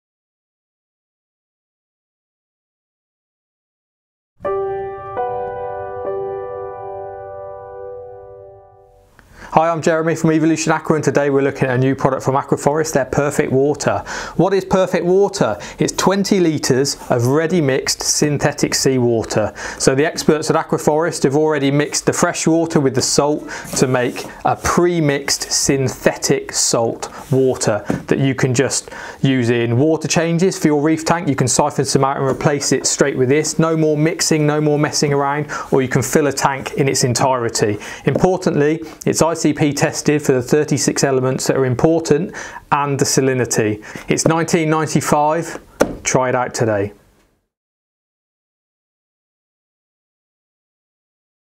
How to use AF Perfect Water (ready mixed reef aquarium saltwater)
How to use AF Perfect Water from Aqua Forest. This ready mixed reef aquarium saltwater is tested by Aqua Forest using their ICP water test machine, ensuring perfect levels of all essential elements and correct salinity.

Available now - 20 litres for £19.99 from your Evolution Aqua dealer.